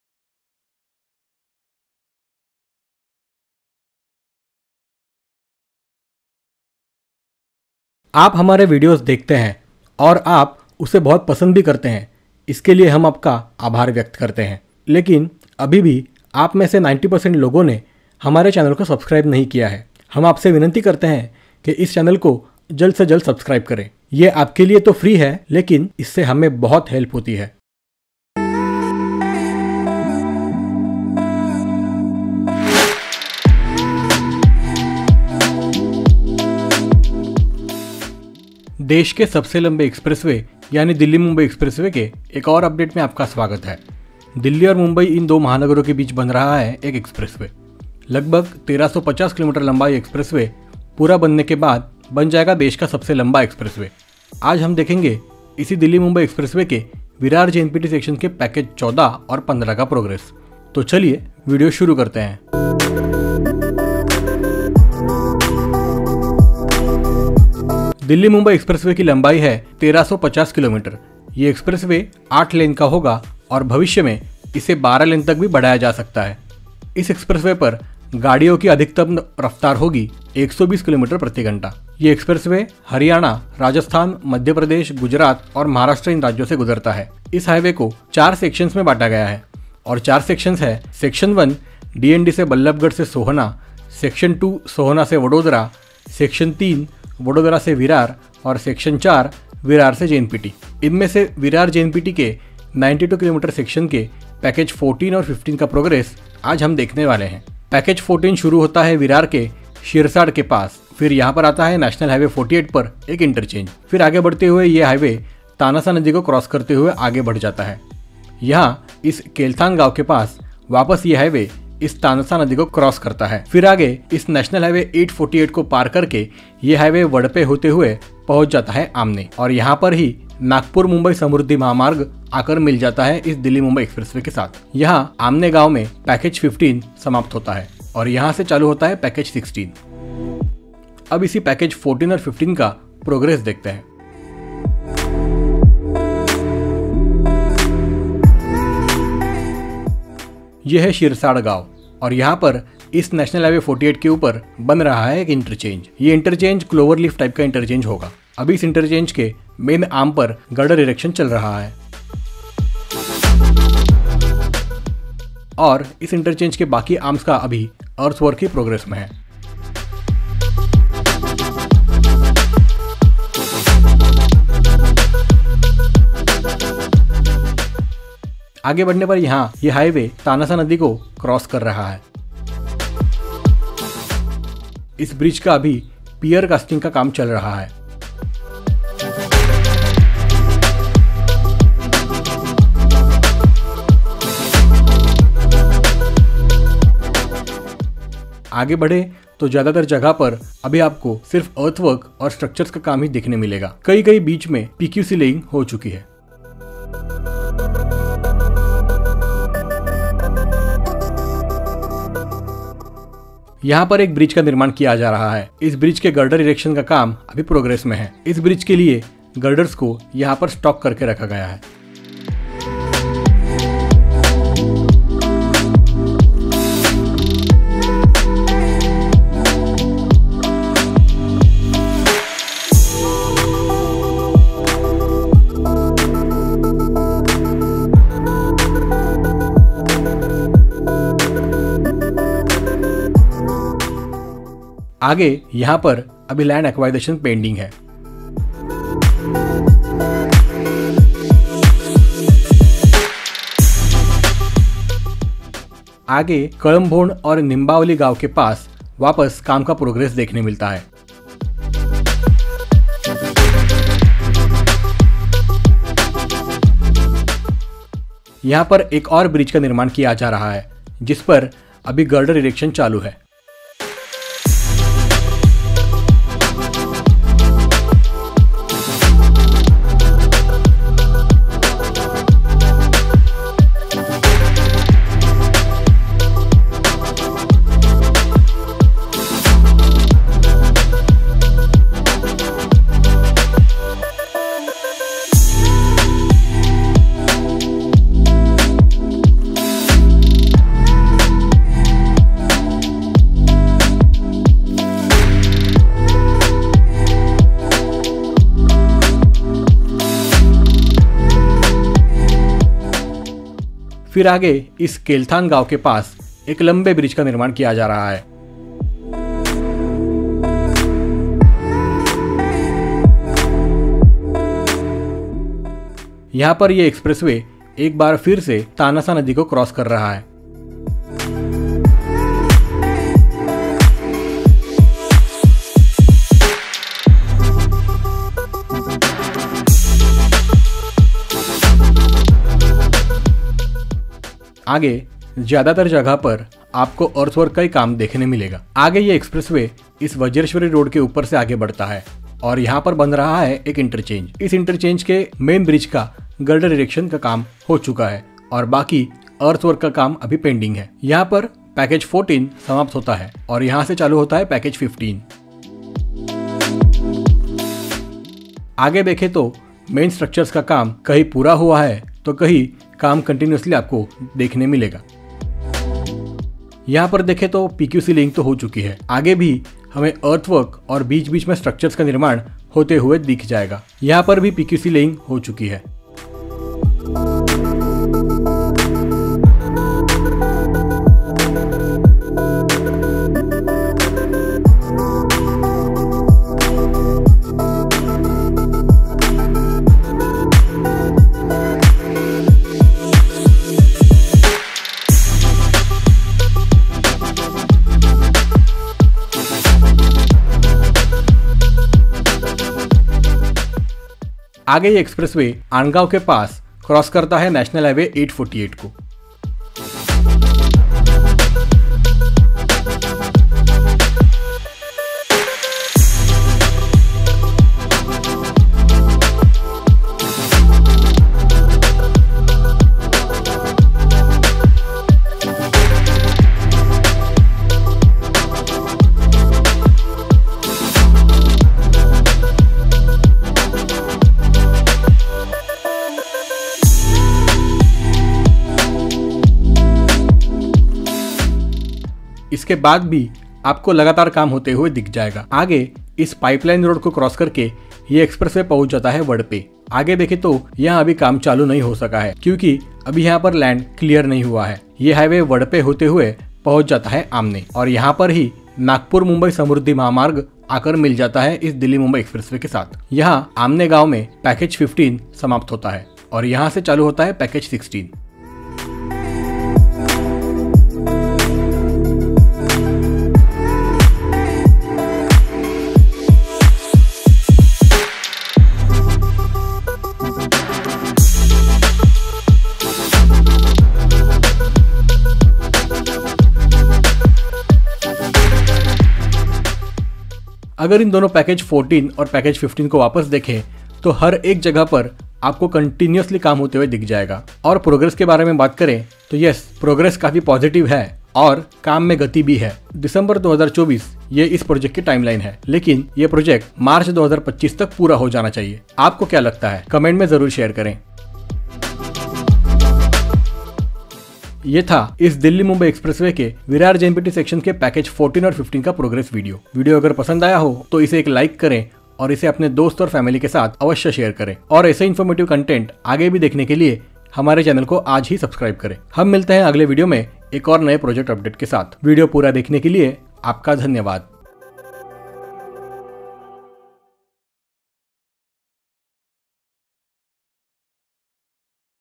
Delhi Mumbai Expressway Maharashtra Update | Virar JNPT Package 14 & 15 Progress
In This Video You will find the progress update of Delhi Mumbai Expressway Virar-JNPT Section Package 14 progress and Delhi Mumbai Expressway Virar-JNPT Section Package 14 progress.

The Delhi–Mumbai Expressway is a 1,350 km long, 8-lane wide (expandable to 12) under-construction access-controlled expressway connecting India's national capital Delhi with its financial capital Mumbai.

Other Infrastructure Updates-